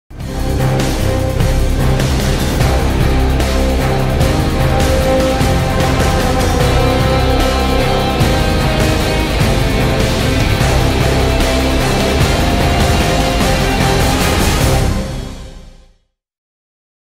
2M company Logo animation HD
a 3d logo animation opener for a company in India